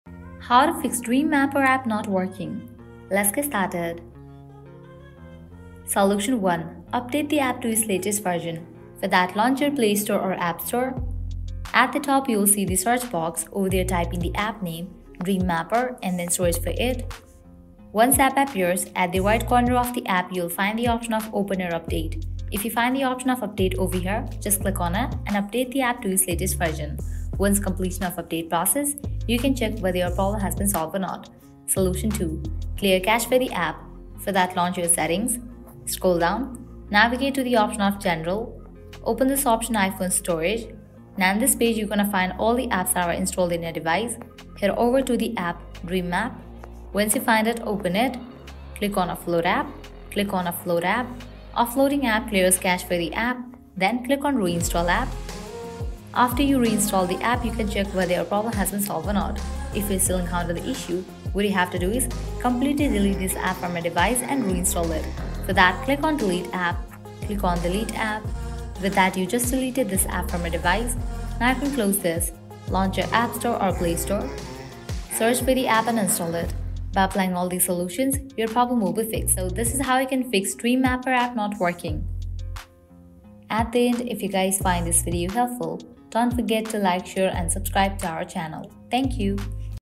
DreamMapper App Not Working: How to Fix DreamMapper App Not Working
Encountering issues with the DreamMapper app can disrupt your sleep apnea therapy monitoring and tracking. This guide provides troubleshooting steps to help you resolve common problems and get the DreamMapper app working smoothly again.

In this guide:
0:00 - Introduction
0:05 - DreamMapper App Not Working: How to Fix DreamMapper App Not Working

Learn how to troubleshoot and fix issues with the DreamMapper app with ease using this comprehensive guide. The guide covers several troubleshooting steps, including checking internet connectivity, updating the app to the latest version, and ensuring compatibility with your device's operating system. Additionally, it provides instructions on restarting the app, clearing cache and data, and reinstalling the app if necessary. Furthermore, you'll find tips on contacting DreamMapper support for further assistance if the issue persists or if you encounter more complex problems. By following these comprehensive steps, you can overcome issues with the DreamMapper app and continue monitoring your sleep apnea therapy effectively.

Fix issues with the DreamMapper app and resume tracking your sleep apnea therapy with confidence using this troubleshooting guide!